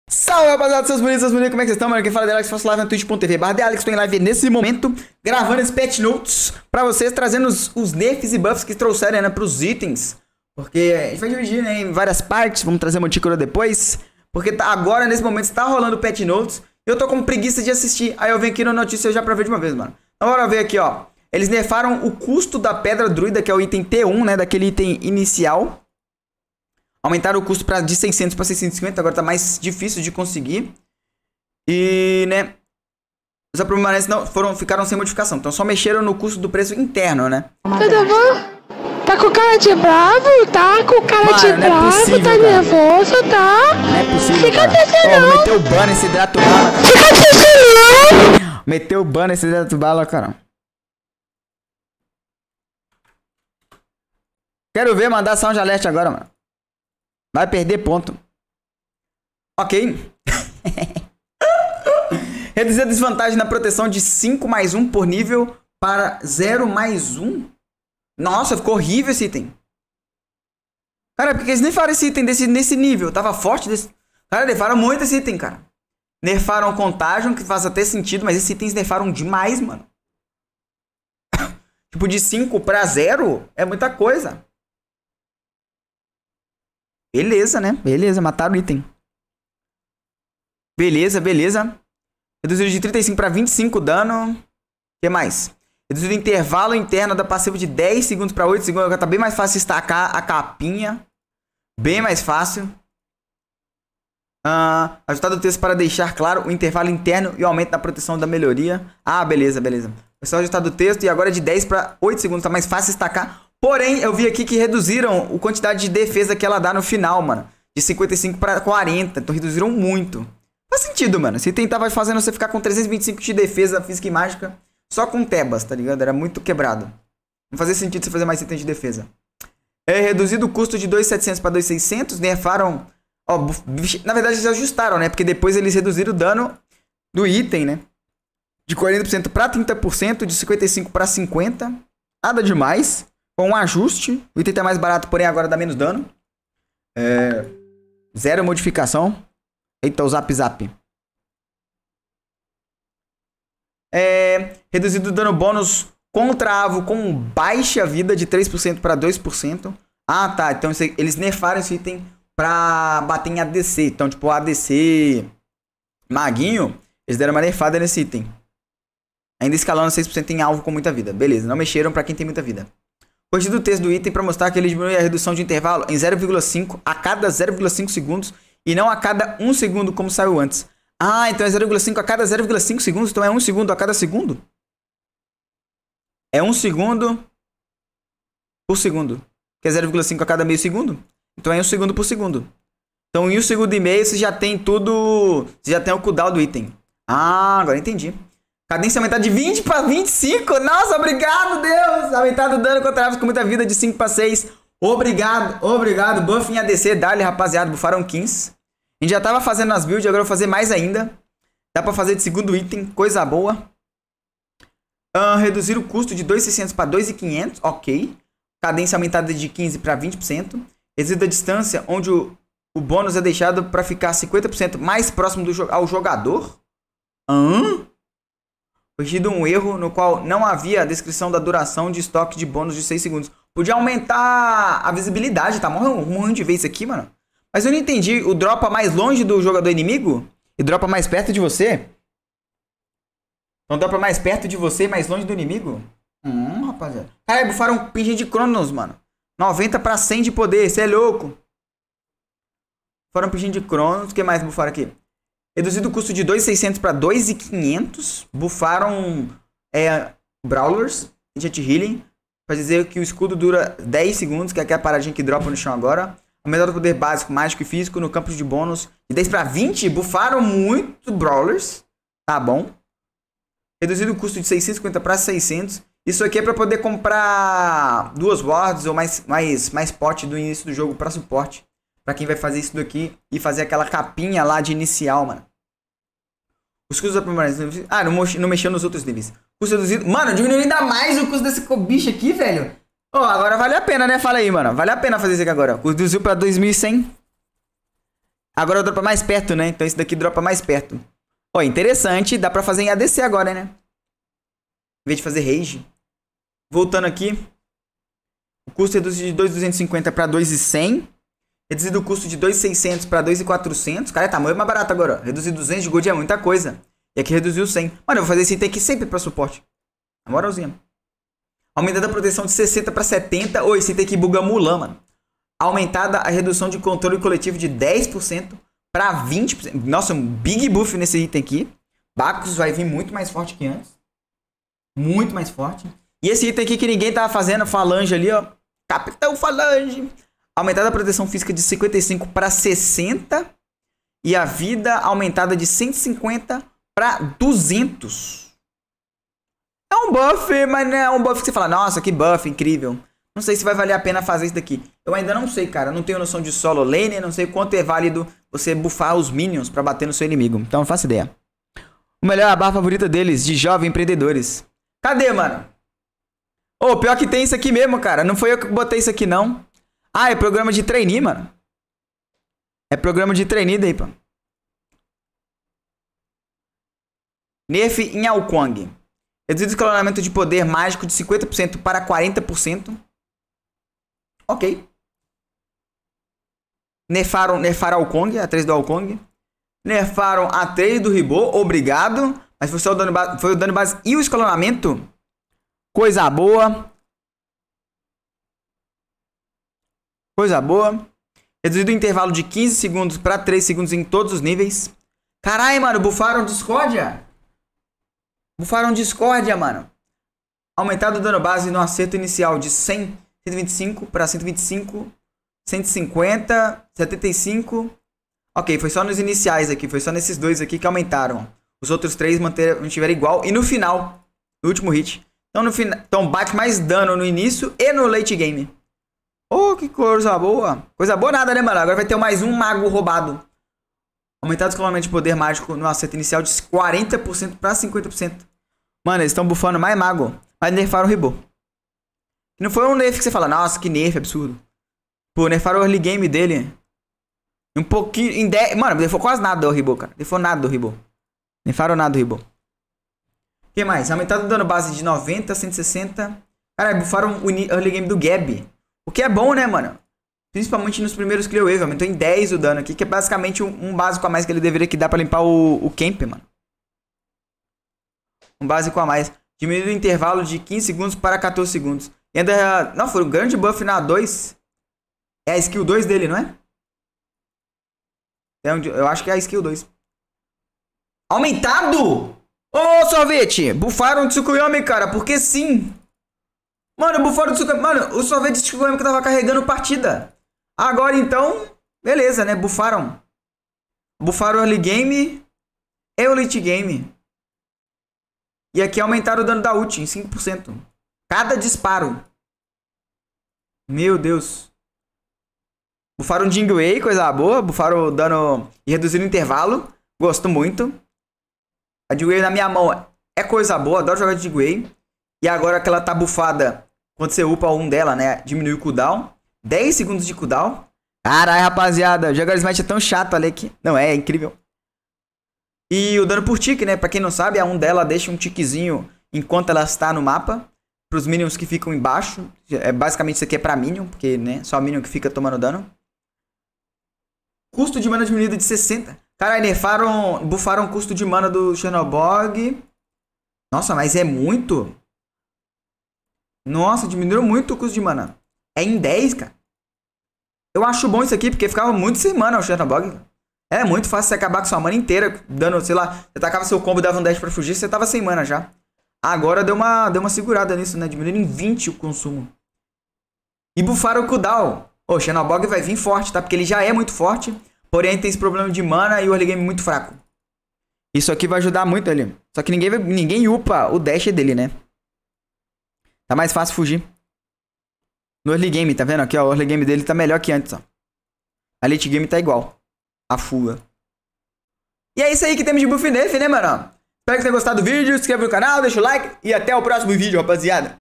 O que mudou? ITENS E DEUSES: BUFFS, NERFS, AJUSTES | Patch Notes 10.2, SMITE BR
(lives todo dia sim e dia não) a partir das 14h!

CREDITOS PROS SEUS JOGOS E DAR

UMA MORAL PRO CANAL? NA PORTAL DOS CRÉDITOS ISSO É POSSIVEL ACESSANDO